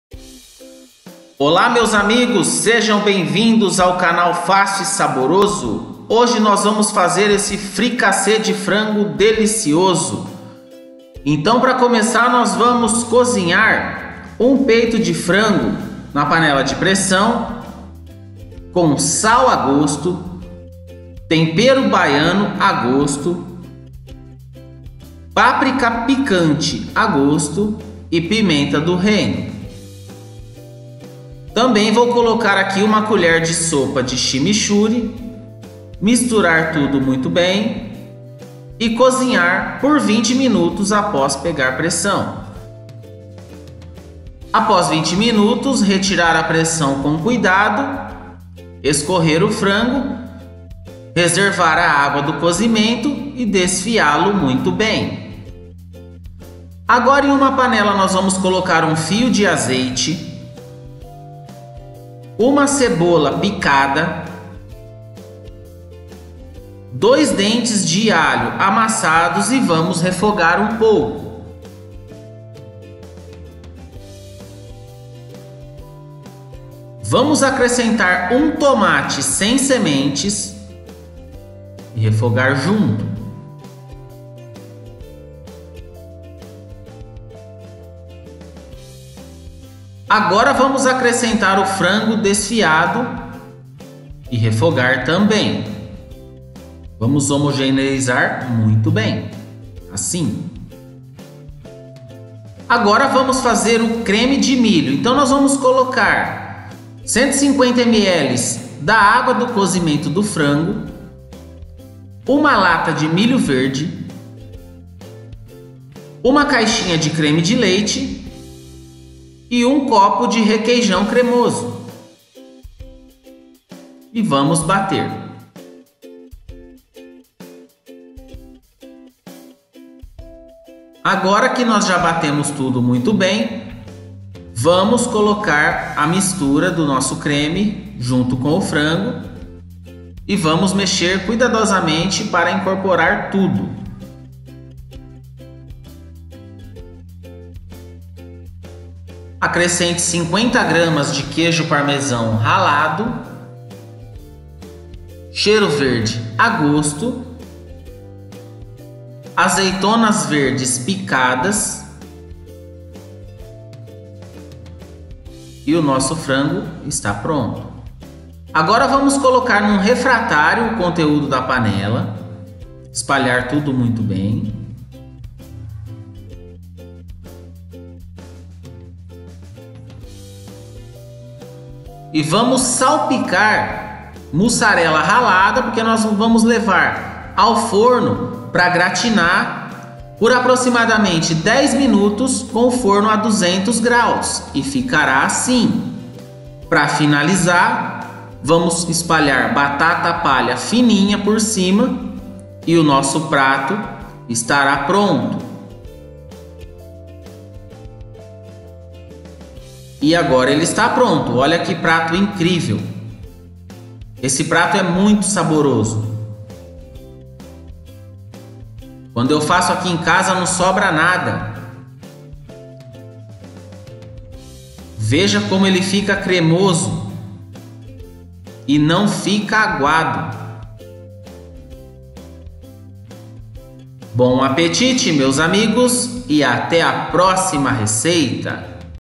FRICASSÉ DE FRANGO - Uma receita incrivelmente Fácil e Saborosa
INGREDIENTES:
1 peito de frango
Sal a gosto
Tempero baiano
Páprica picante a gosto
Pimenta do reino a gosto
1 colher de sopa de chimichurri
Azeite
1 cebola
2 dentes de alho
1 tomate
150ml de água do cozimento do frango
1 lata de milho verde
1 caixinha de creme de leite
1 copo de requeijão cremoso
50g de queijo parmesão ralado
Cheiro verde a gosto
Azeitonas verdes
Mussarela ralada
1 pacote de batata palha